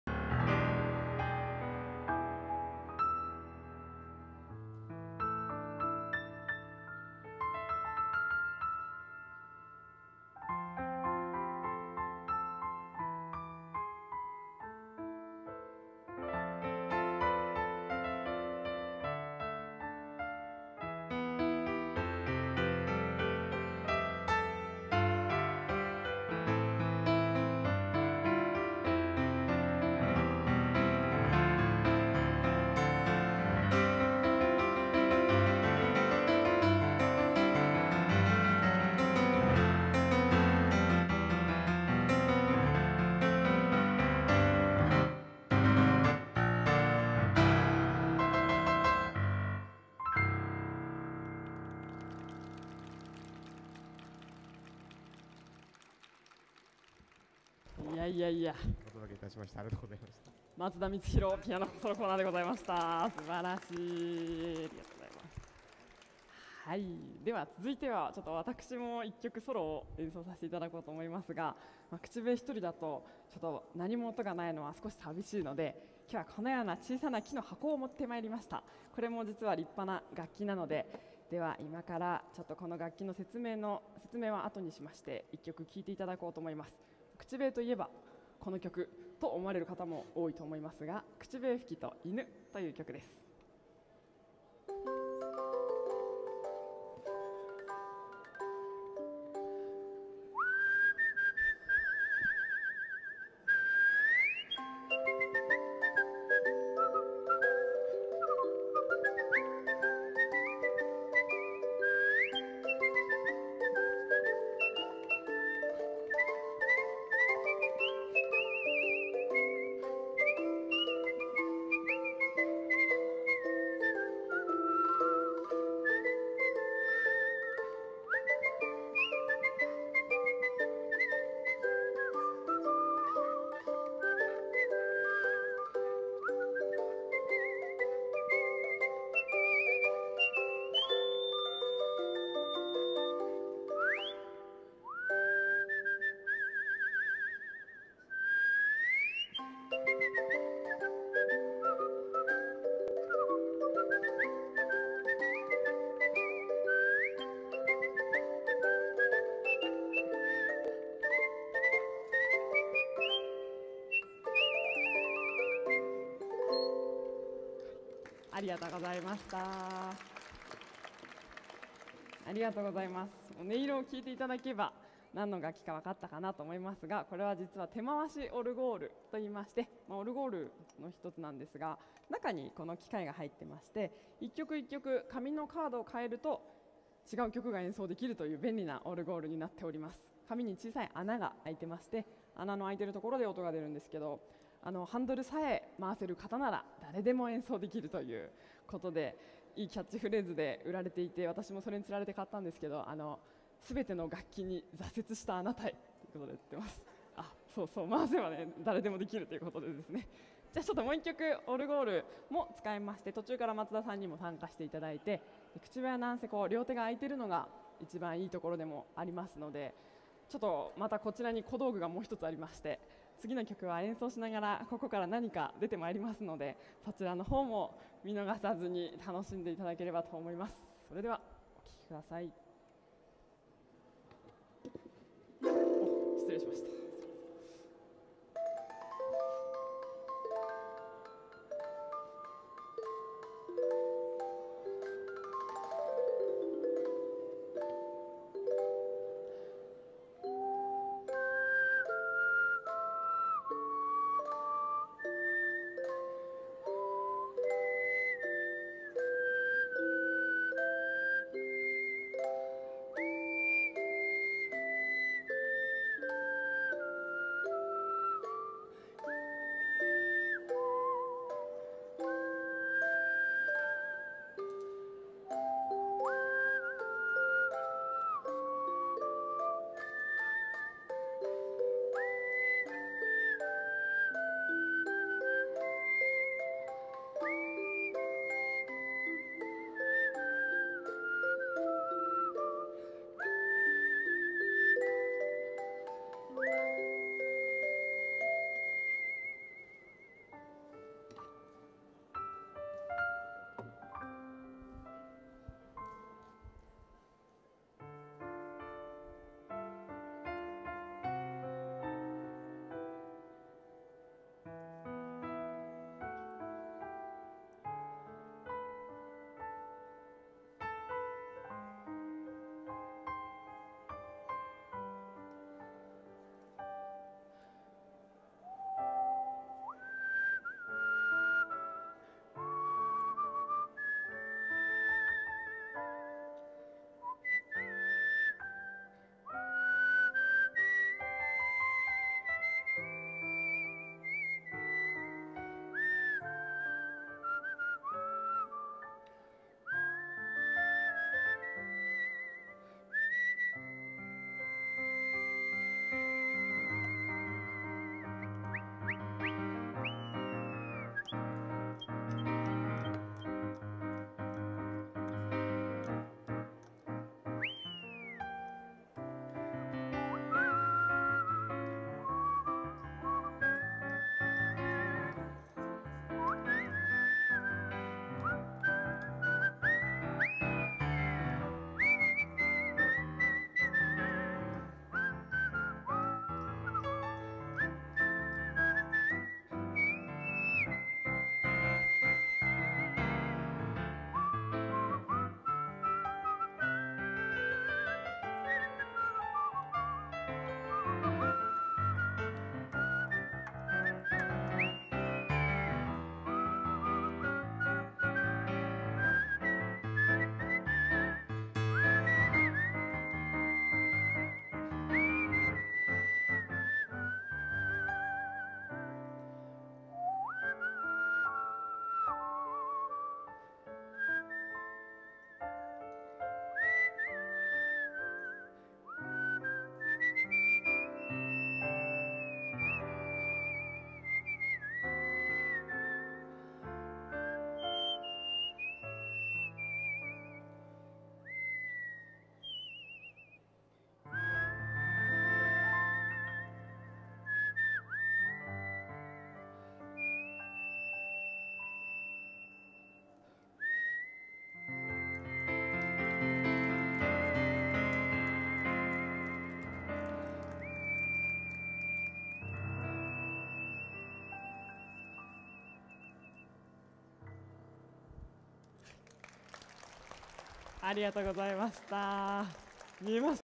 柴田晶子 口笛コンサート (アルヴェ市民活動フェスタ2015)
2015年3月1日
アルヴェ1階　きらめき広場
ピアノ　松田光弘